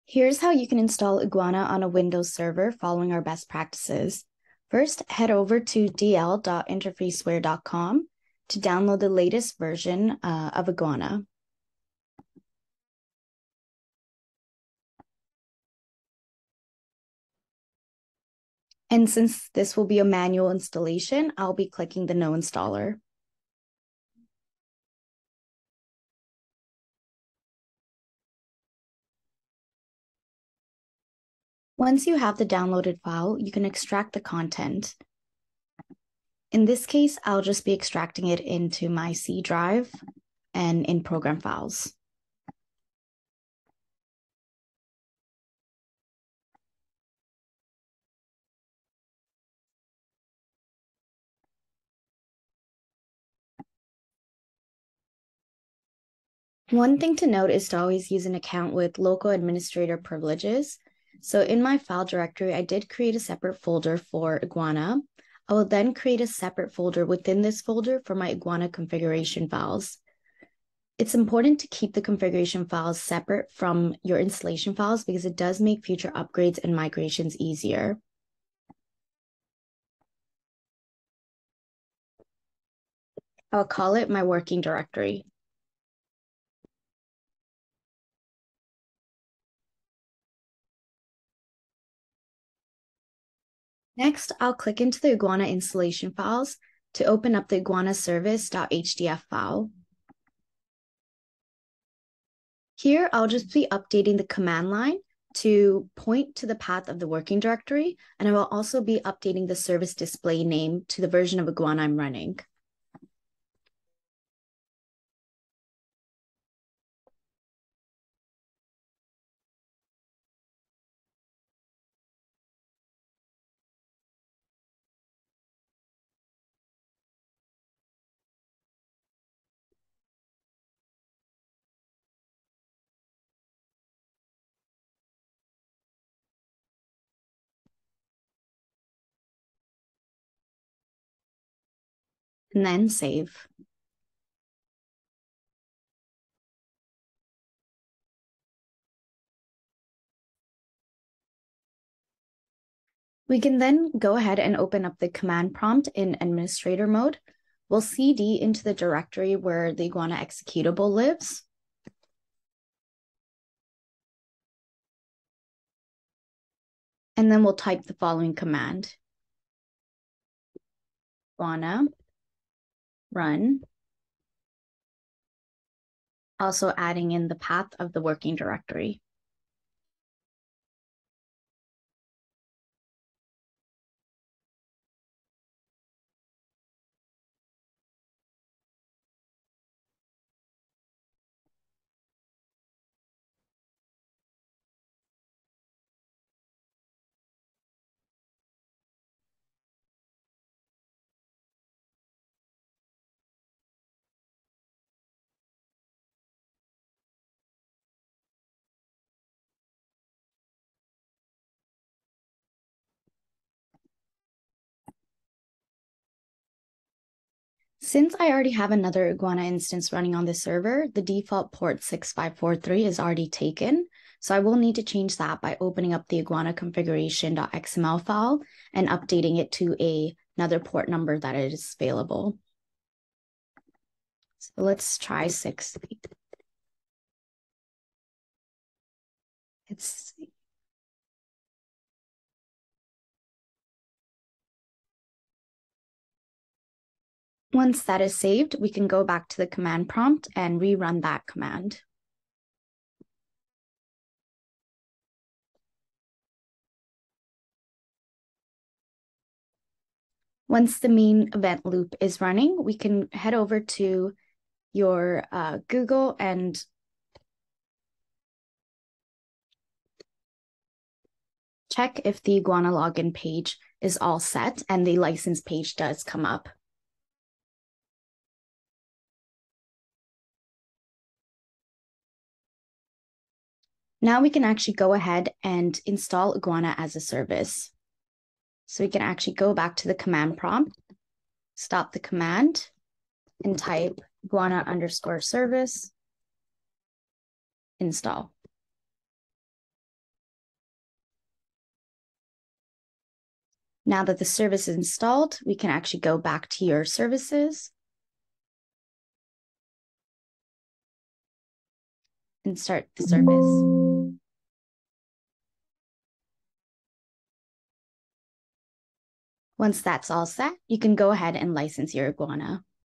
How to Install Iguana on a Windows Server
Learn how to install Iguana on a Windows Server.